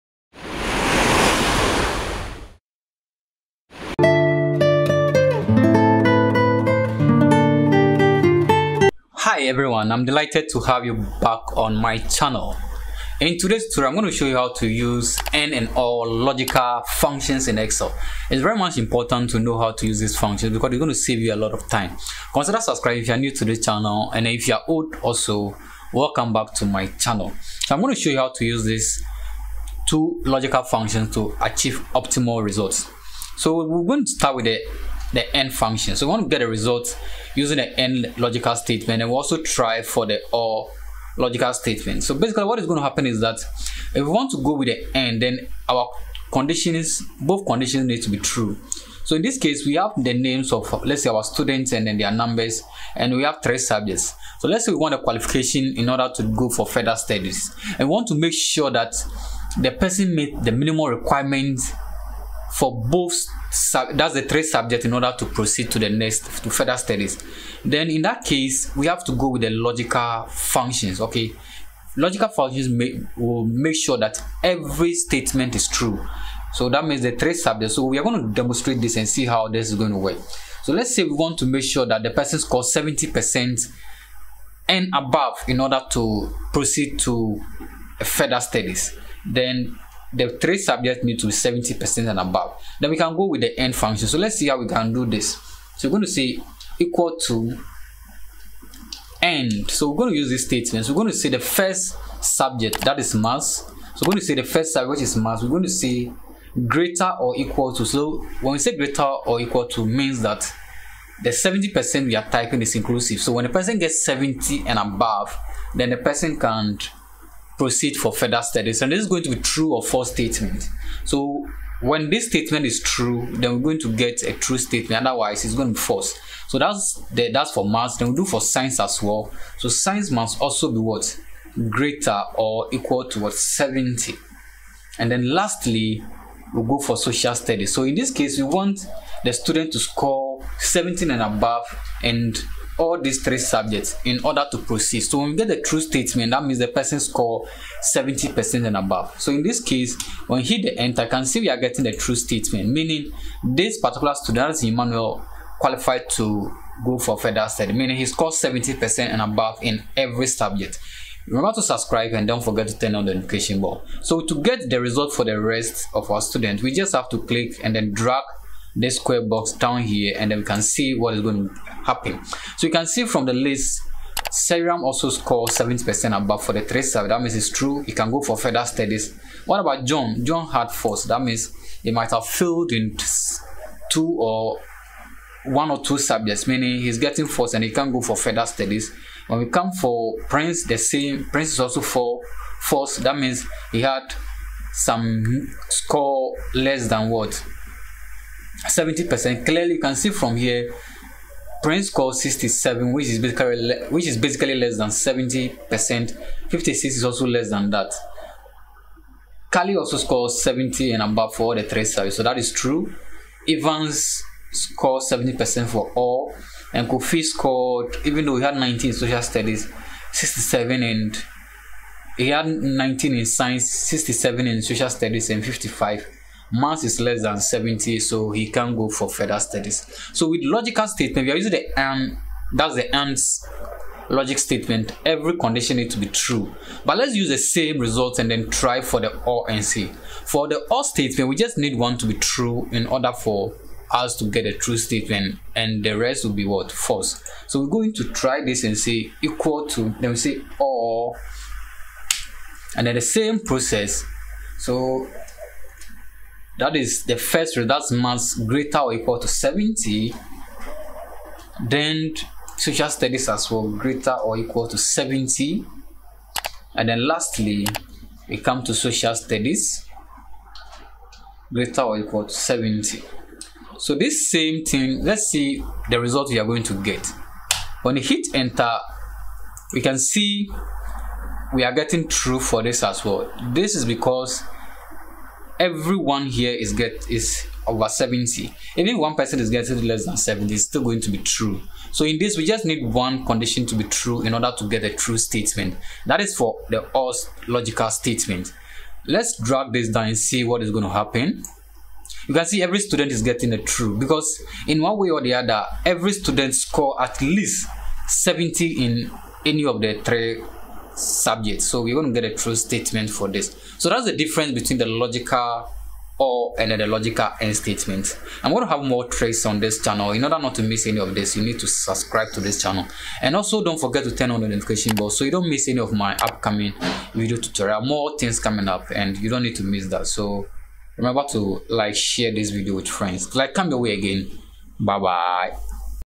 How to use AND & OR logical functions in excel
Hi there, In this tutorial, will be teaching how to use AND & OR logical functions in excel.